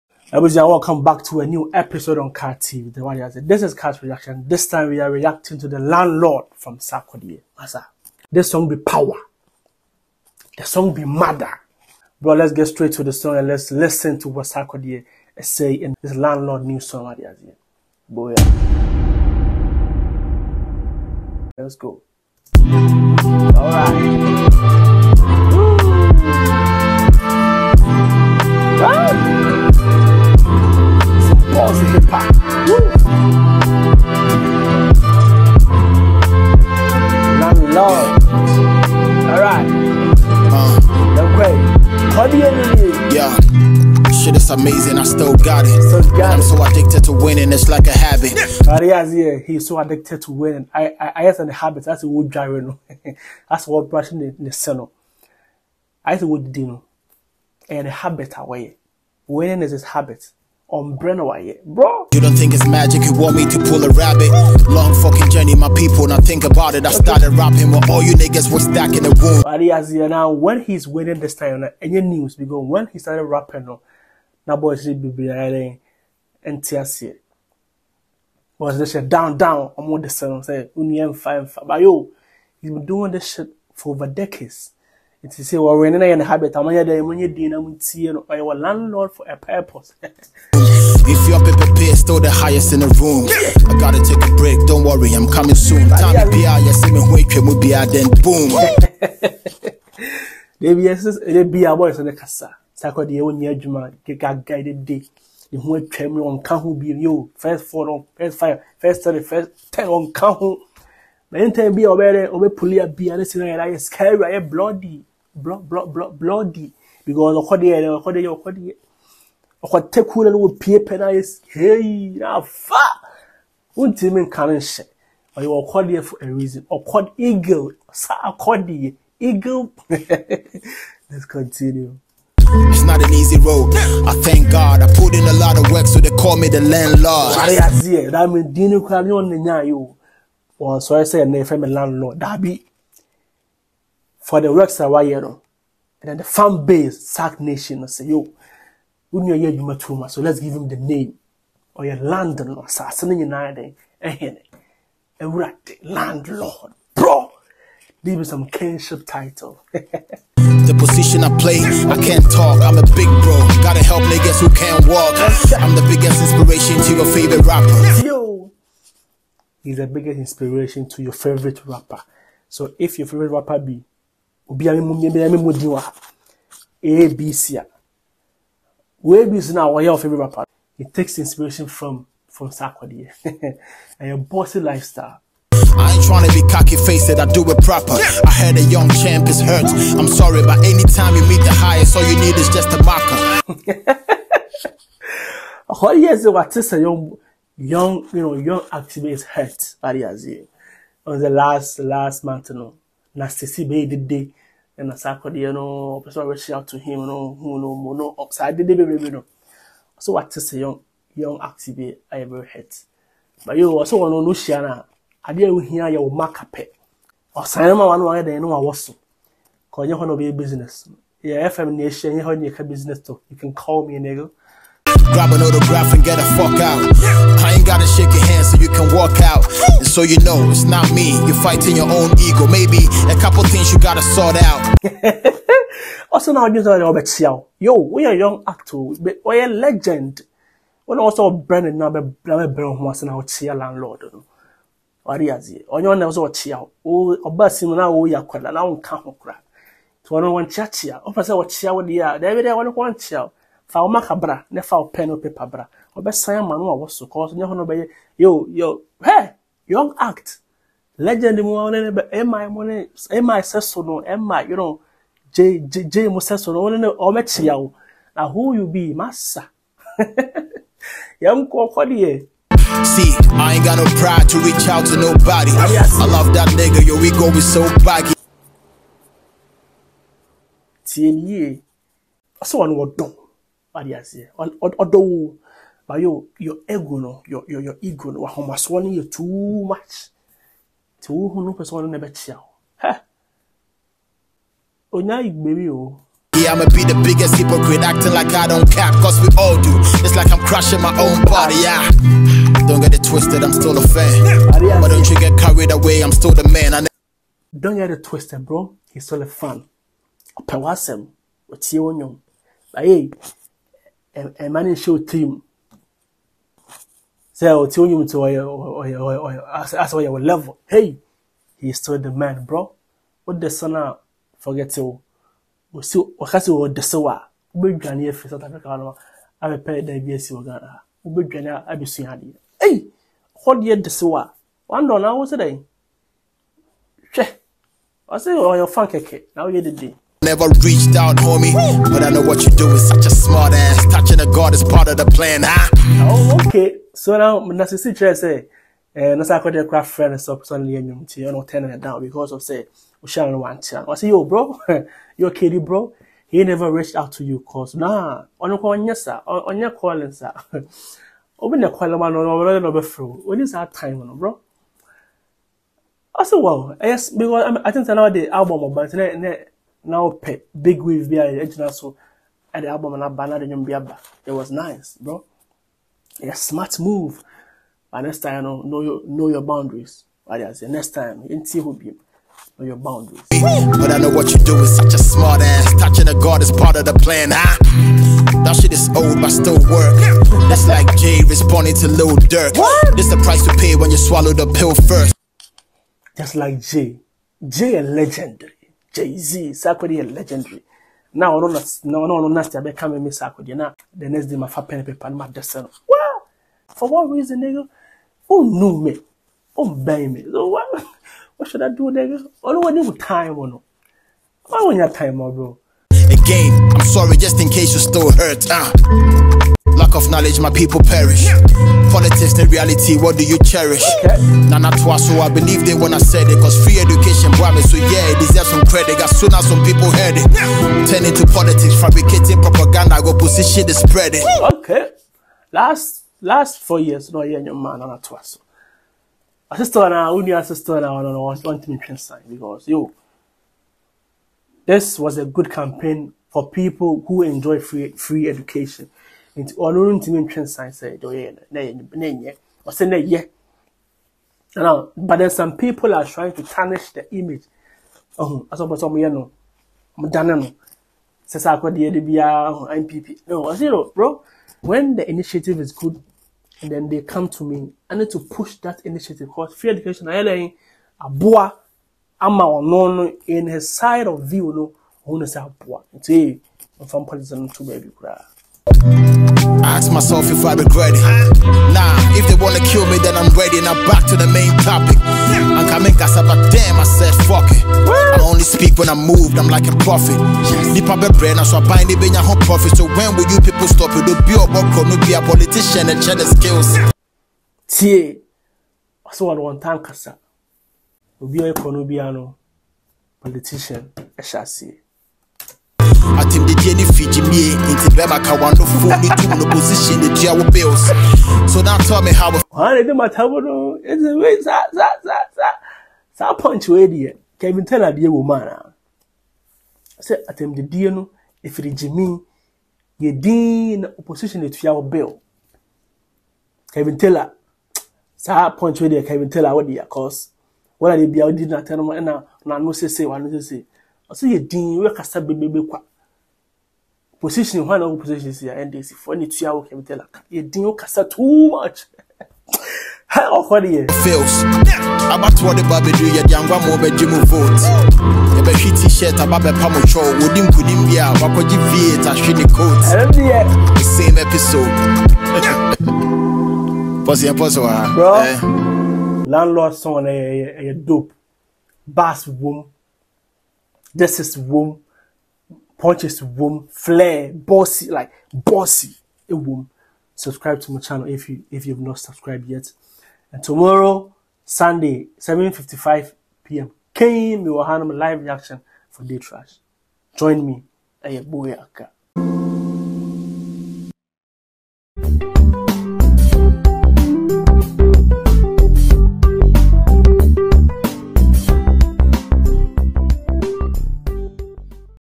Sarkodie Replies Nasty C + Throws Shot At Kevin Taylor In His New Song || Landlord Reaction🔥🇬🇭
Kart Reacting To Sarkodie's  Latest  single